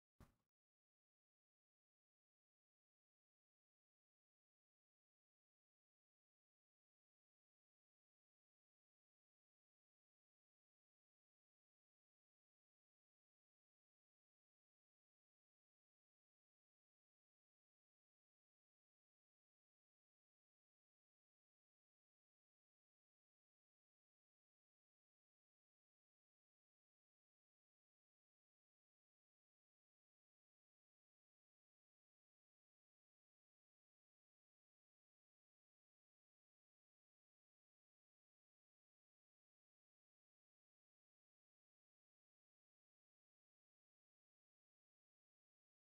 Appetizer Crate Unboxing (Pardon the Quality)
I was going to record this, then I forgot to open up the screen recorder so I just did this; hope it looks alright still.